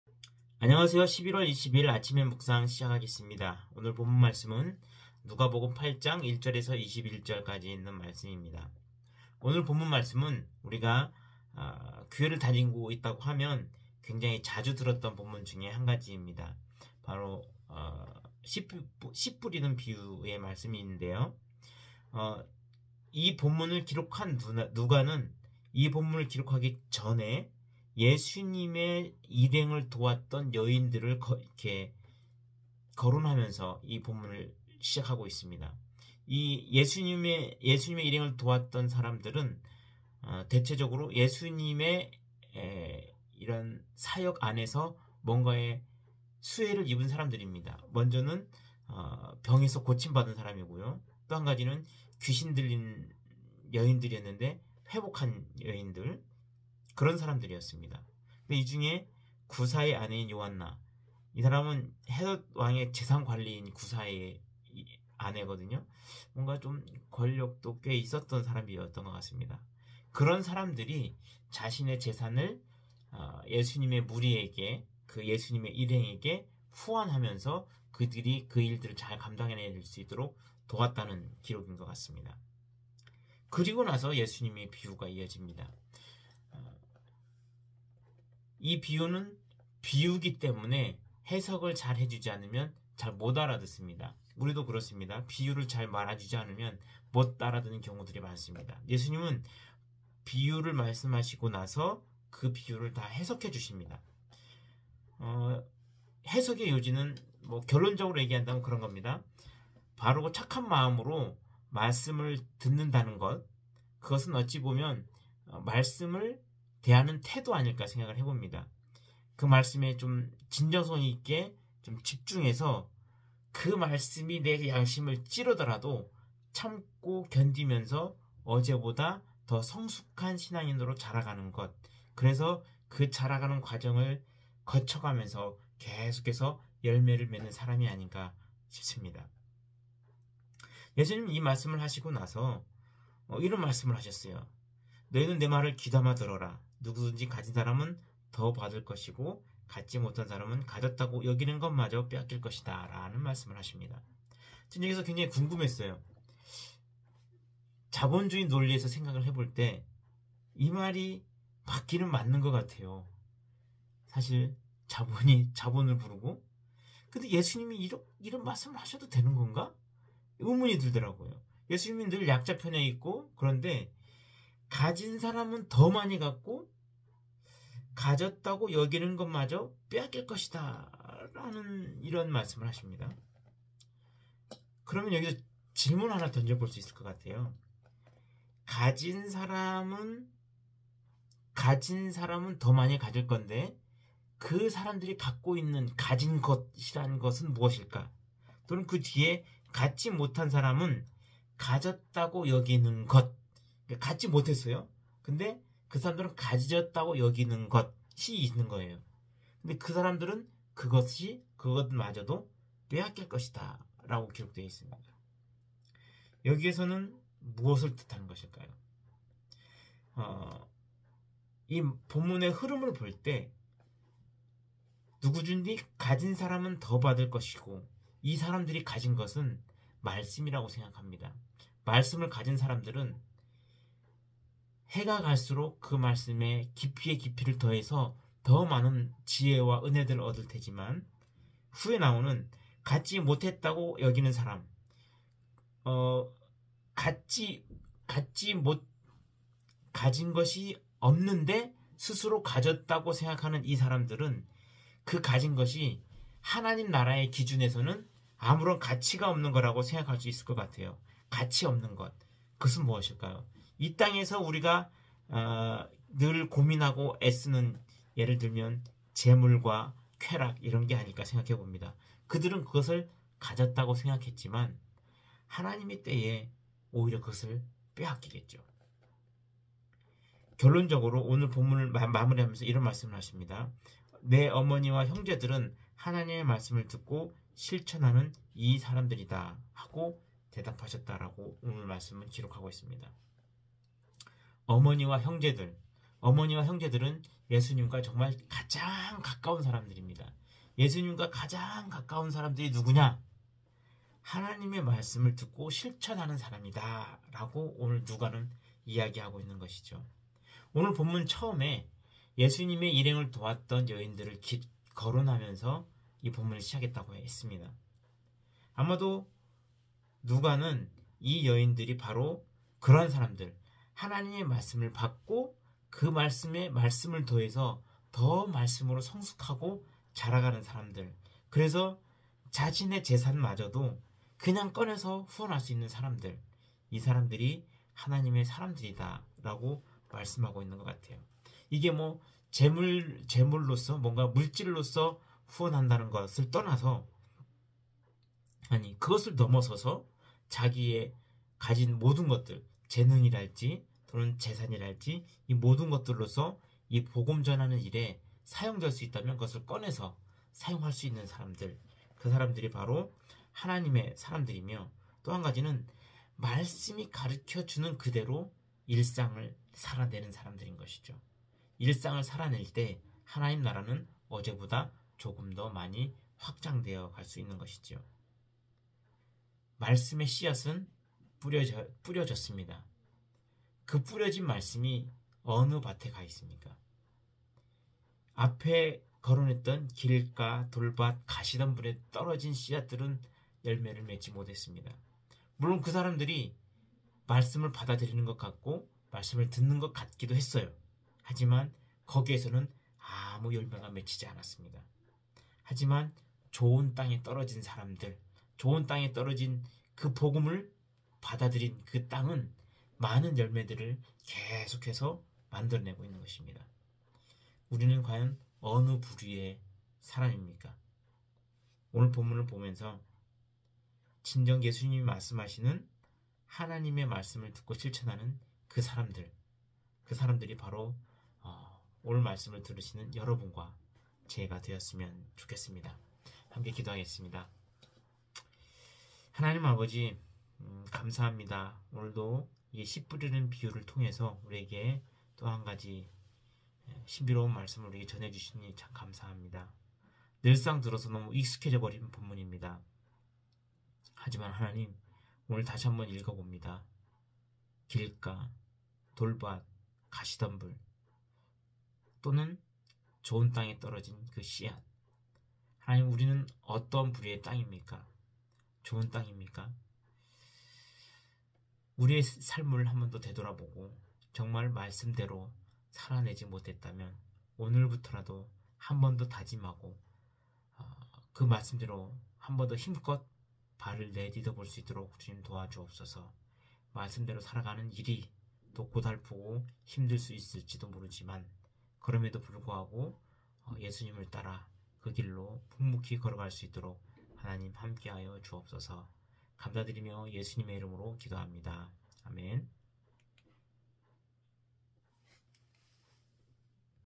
11/20(월) [아침N묵상] | 와플터치QT | 누가복음 8:1-21 (김제룡)
1그 후에 예수님은 여러 도시와 마을을 다니시며 하나님의 나라를 선포하시고 기쁜 소식을 전하셨는데 열두 제자도 동행하였다.

2또 악한 귀신들과 여러 가지 병에서 고침을 받은 여자들도 예수님과 동행하였다. 그들 중에는 일곱 귀신이 나간 막달라 마리아,

3헤롯왕의 재산 관리인 구사의 아내 요안나, 수산나, 그 밖에 다른 여자들도 여럿 있었다. 그들은 자기들의 재산을 바쳐 예수님의 일행을 도왔다.

4사람들이 여러 마을에서 모여들어 마침내 큰 군중을 이루었을 때 예수님은 비유를 들어 이렇게 말씀하셨다.

5한 농부가 들에 나가 씨를 뿌렸다. 그런데 어떤 씨는 길가에 떨어져서 발에 밟히고 새들이 와서 먹어 버렸다. 그리고

6어떤 씨는 돌밭에 떨어져서 싹은 나왔으나 물기가 없어 말라 버렸다.

7또 어떤 씨는 가시덤불에 떨어졌는데 가시나무가 함께 자라 그 기운을 막았다.

8그러나 어떤 씨는 좋은 땅에 떨어져서 잘 자라 100배의 열매를 맺었다. 예수님은 이 말씀을 마치시고 "귀 있는 사람은 잘 들어라" 하고 외치셨다.

9제자들이 이 비유의 뜻을 묻자

10예수님은 이렇게 대답하셨다. 너희에게는 하나님 나라의 비밀을 아는 것이 허락되었으나 다른 사람들에게는 비유로 말한다. 이것은 그들이 보아도 알지 못하고 들어도 깨닫지 못하게 하기 위해서이다.

11이 비유의 뜻은 이렇다 : 씨는 하나님의 말씀이다.

12길가에 떨어진 씨는 사람들이 말씀을 듣기는 하였으나 그들이 믿고 구원받지 못하도록 마귀가 와서 그 말씀을 빼앗아 가는 것을 가리킨다.

13돌밭에 떨어진 씨는 말씀을 들을 때 기쁨으로 받아들이지만 뿌리가 없으므로 잠시 믿다가 시험을 받으면 떨어져 나가는 사람을 말한다.

14또 가시덤불 속에 떨어진 씨는 말씀은 듣지만 살아가는 동안에 세상의 걱정과 재물에 얽매이고 쾌락에 빠져 말씀대로 생활하지 못하는 사람이다.

15그러나 좋은 땅에 뿌려진 씨는 바르고 착한 마음으로 말씀을 듣고 그 말씀을 잘 간직하여 인내로 열매를 맺는 사람을 가리킨다.

16"등불을 켜서 그릇으로 덮어 두거나 침대 아래 둘 사람은 아무도 없다. 오히려 그것을 등잔대 위에 올려놓아 들어오는 사람이 그 빛을 볼 수 있도록 하지 않겠느냐?

17감추어진 것은 나타나기 마련이고 비밀은 알려지고 드러나기 마련이다.

18그러므로 너희는 내 말을 귀담아 들어라. 누구든지 가진 사람은 더 받을 것이고 갖지 못한 사람은 가졌다고 여기는 것마저 빼앗길 것이다."

19예수님의 어머니와 형제들이 예수님을 만나러 왔으나 군중들 때문에 만날 수가 없었다.

20그래서 어떤 사람이 예수님께 "선생님, 어머니와 형제분들이 선생님을 만나 뵈려고 밖에서 기다립니다" 하였다.

21그러나 예수님은 그들에게 "내 어머니와 형제들은 하나님의 말씀을 듣고 실천하는 이 사람들이다" 하고 대답하셨다.